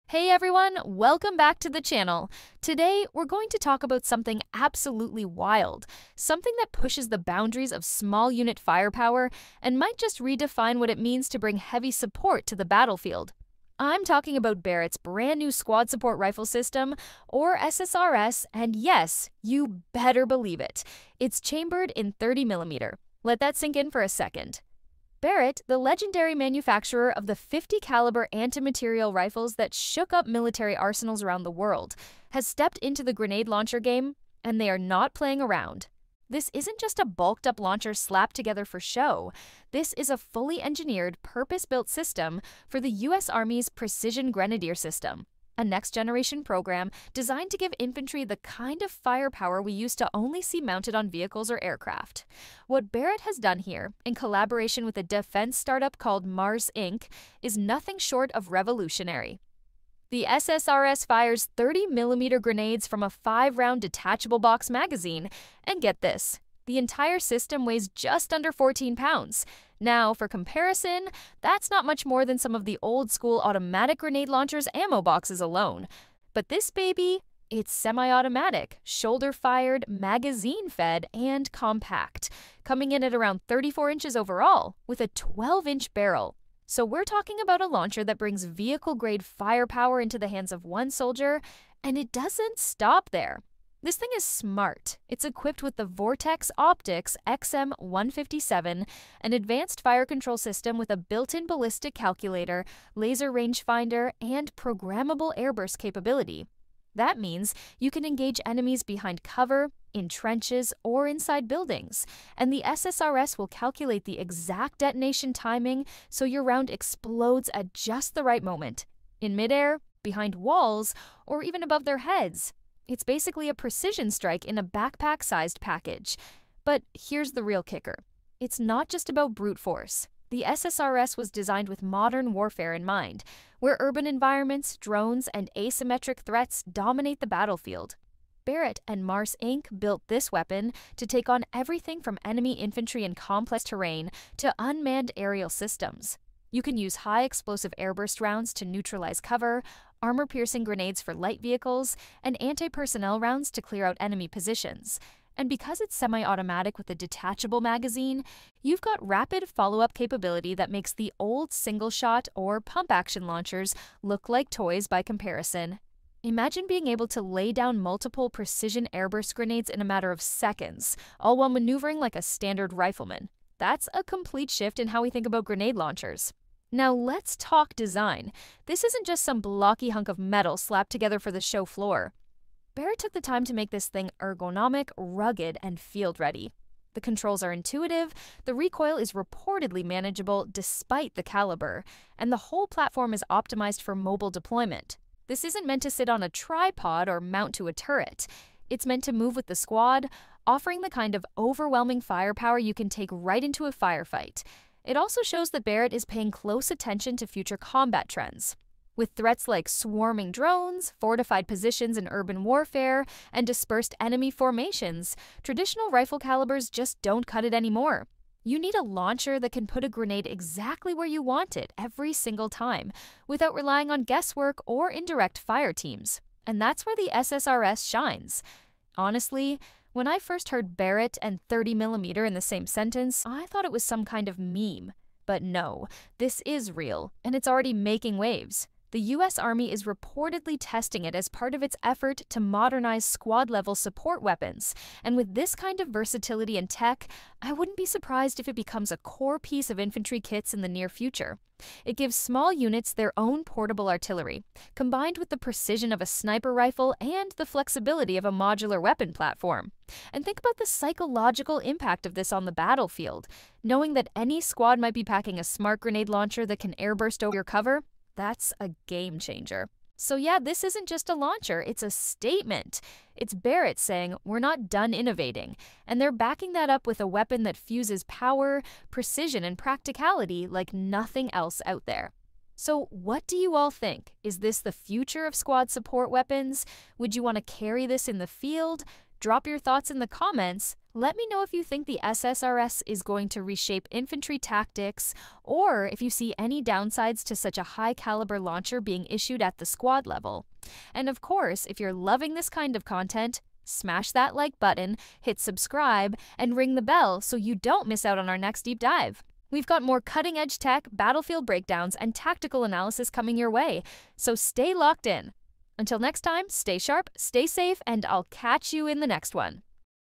Barrett’s 30mm Grenade Launcher Is INSANE – The Future of Infantry Firepower!
Hey everyone, welcome back to the channel! In today’s episode, we’re diving deep into the jaw-dropping new weapon system from the legendary firearms manufacturer Barrett—the 30mm Squad Support Rifle System (SSRS). Yes, you heard that right—thirty millimeter. This isn’t science fiction, this is real-world, battlefield-ready tech designed for the U.S. Army’s Precision Grenadier System. In this video, we break down the specs, tech, and tactical advantages of Barrett’s semi-automatic smart grenade launcher that brings vehicle-level firepower straight into the hands of a single soldier. We’ll explore its programmable airburst capability, integration with the Vortex XM157 optic, its anti-drone potential, and the massive impact it could have on modern infantry tactics. Whether you’re into military technology, future weapons, or just love geeking out over next-gen gear, this one’s for you.